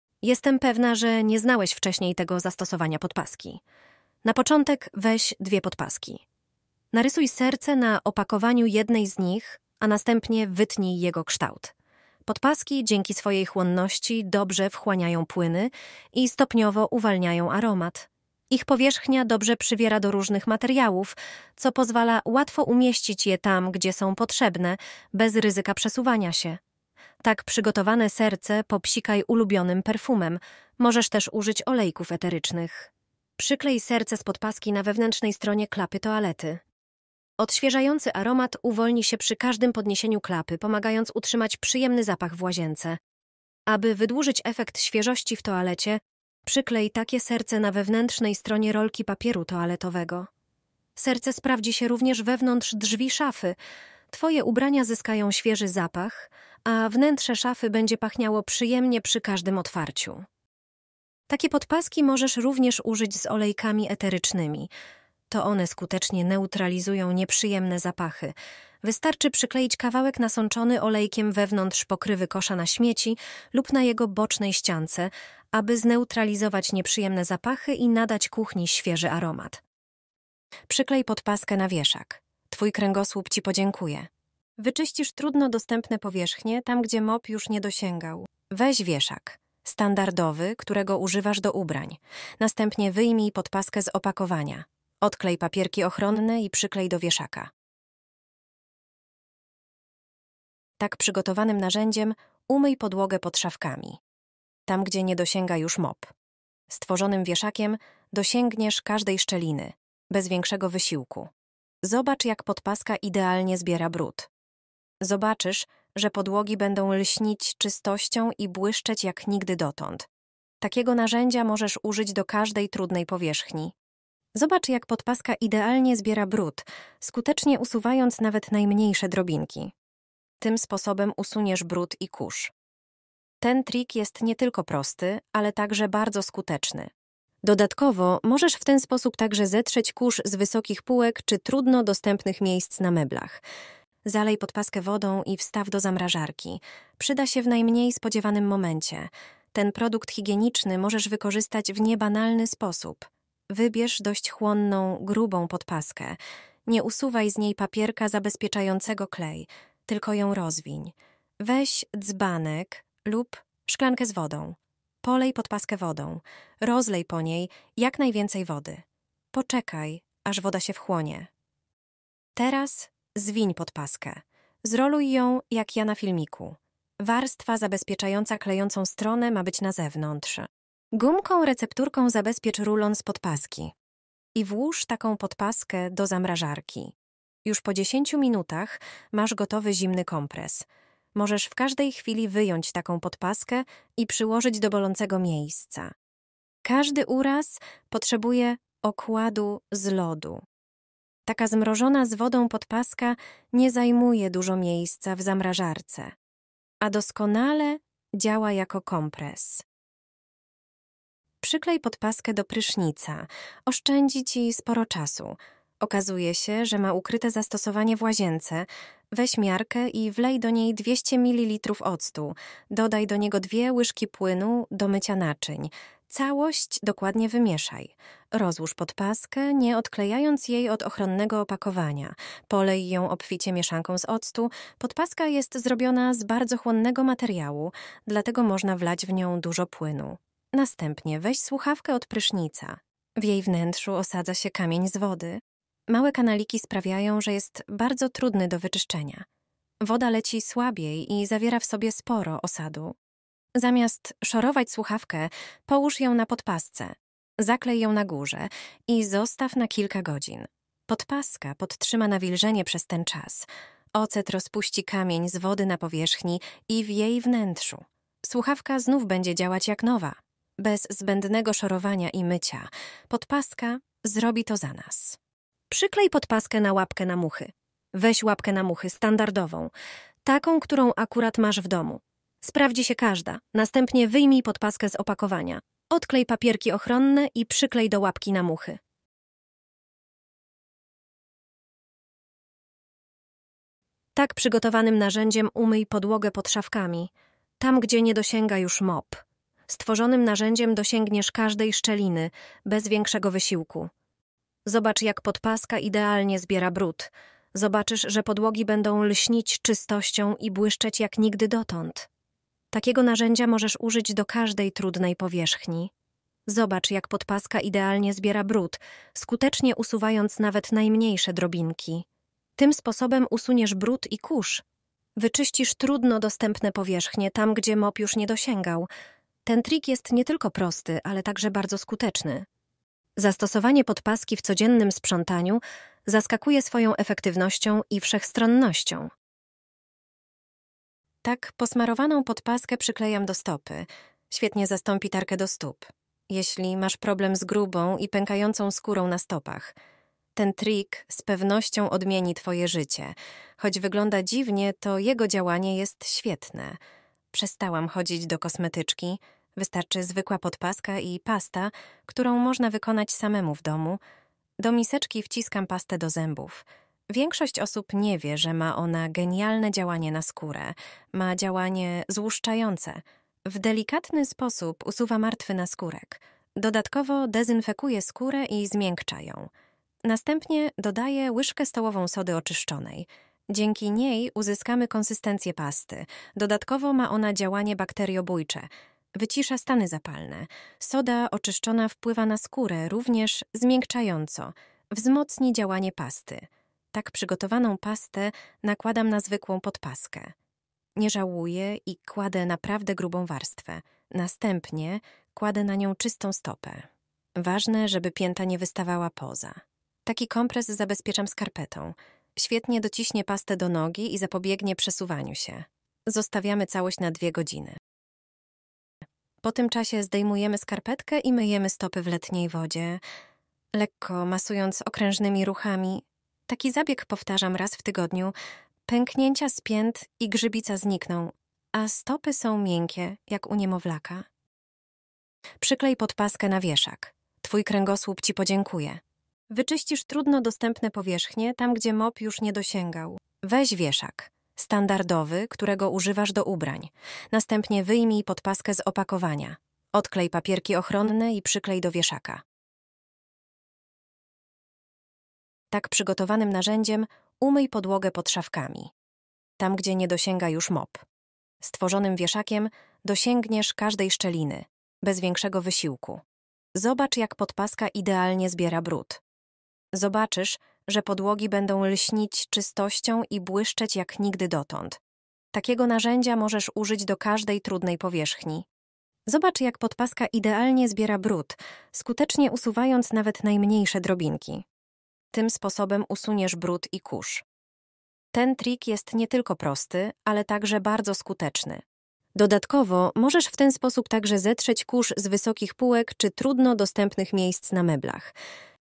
Spryskaj podpaskę ulubionym perfumem. Tego na pewno nie wiedziałeś
Poznaj kreatywny sposób na wykorzystanie podpaski. Dzięki chłonności podpaski zapach ulubionych perfum lub olejków eterycznych uwalnia się stopniowo.